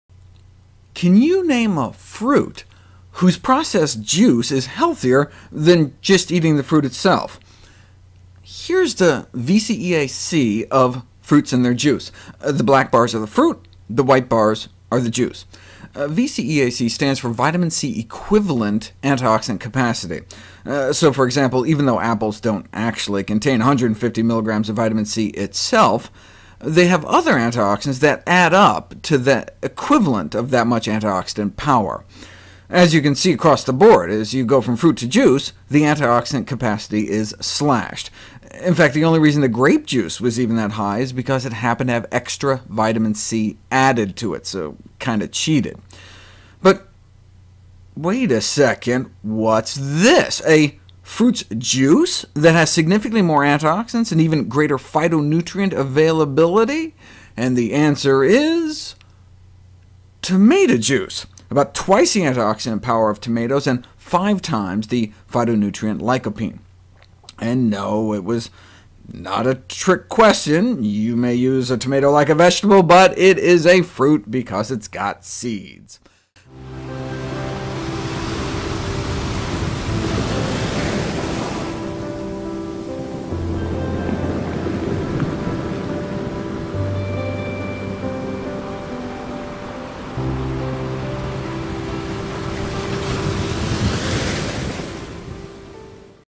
Is Tomato Juice Healthier Than Whole Tomatos?
You may be tempted into thinking that since fruit juice comes straight from the fruit, they must be nutritionally equivalent. But such thinking is wrong.

The edible skins of many of the world's healthiest fruits are all sites of important biological activity in the life of the fruit. The skin is one of the places where the fruit interacts with sunlight, and forms a variety of colored pigments that absorb different wavelengths of light. These pigments, including carotenoids and flavonoids, are well researched as nutrients that protect our health and nourishment.

Tomatos seem to be an exception to this rule. Tomato juice has about twice the amount of antioxidants and five times the phytonutrient lycopene as compared with whole tomatos.

It is not a substitute for professional medical advice, diagnosis or treatment.